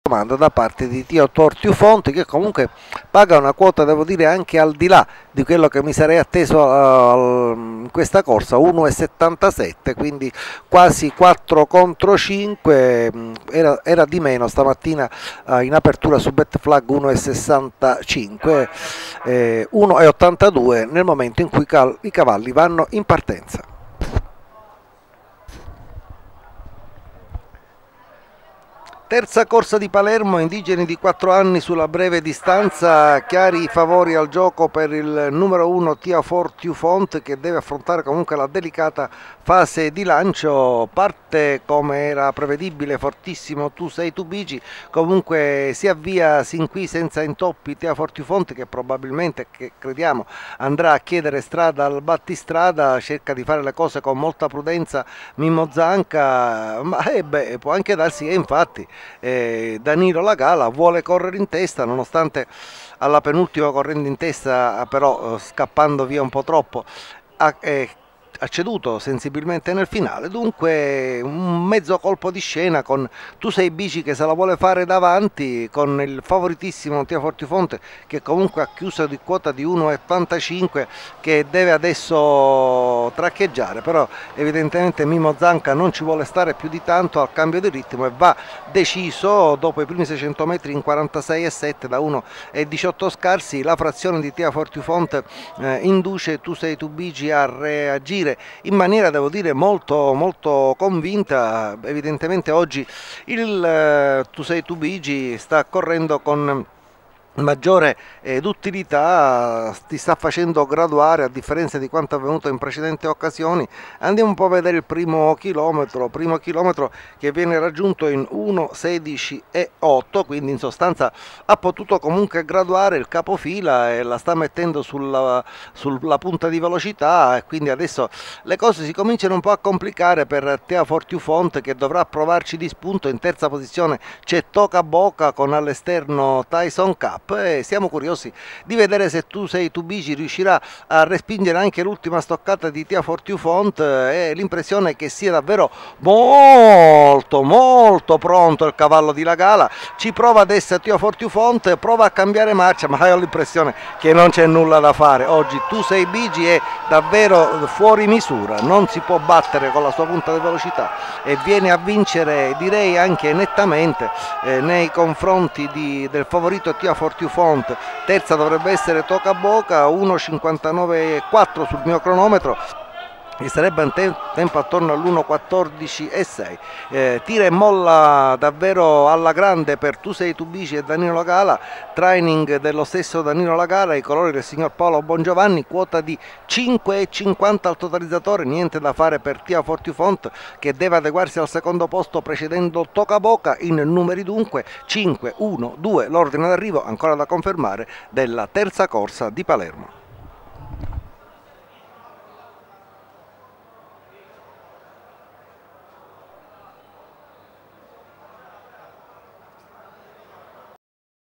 3° CORSA del 23 NOVEMBRE 2015
Premio SCANDICCI  Mt.1600 (3 Anni)  Cronaca: Pietro Di Salvo